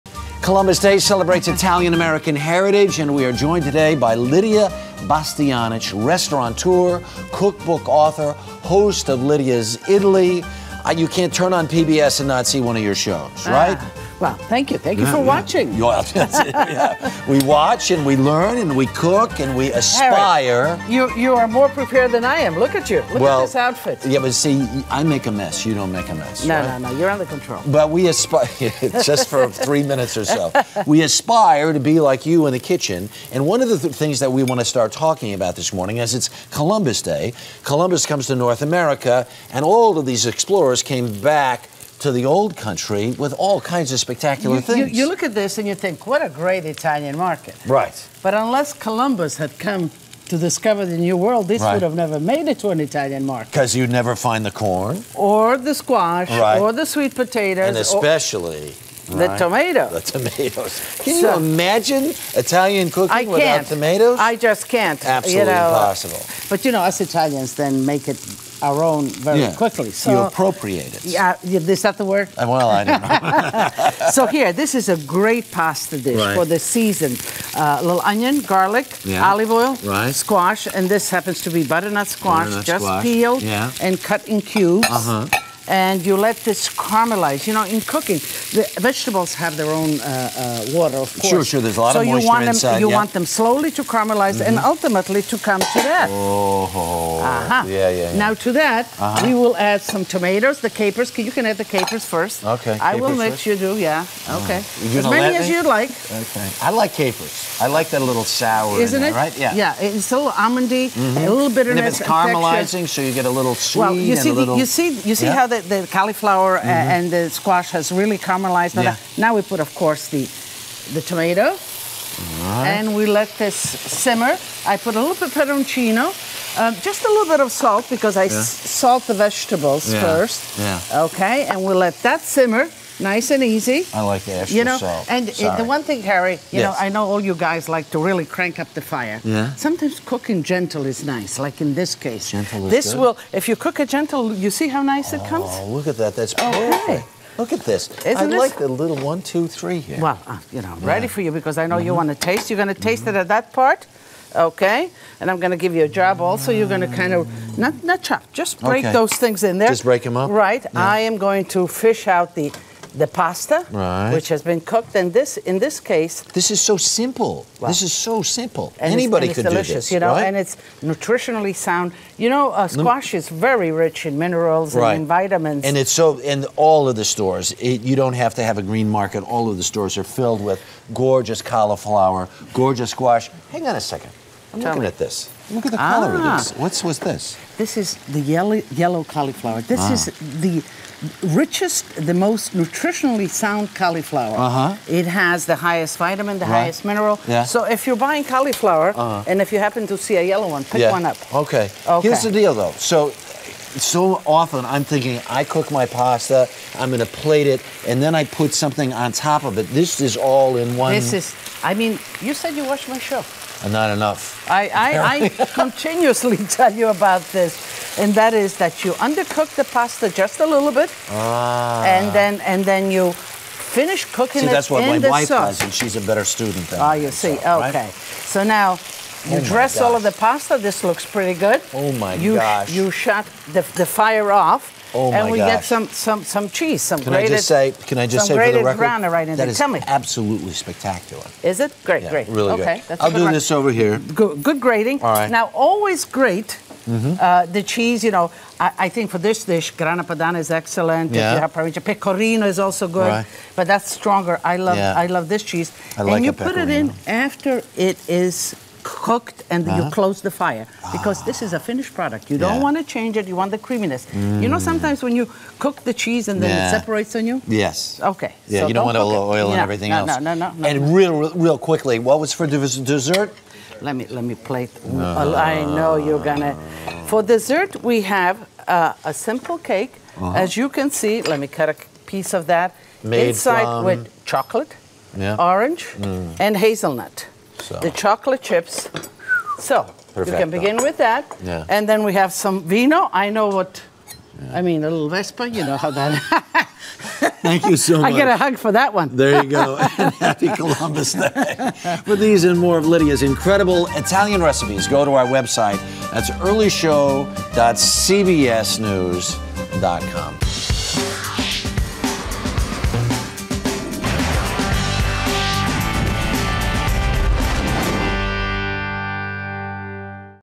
Pasta With American Veggies
Chef Lidia Bastianich uses squash, cauliflower and tomato, vegetables Columbus brought to Europe, for an Italo-American celebration meal.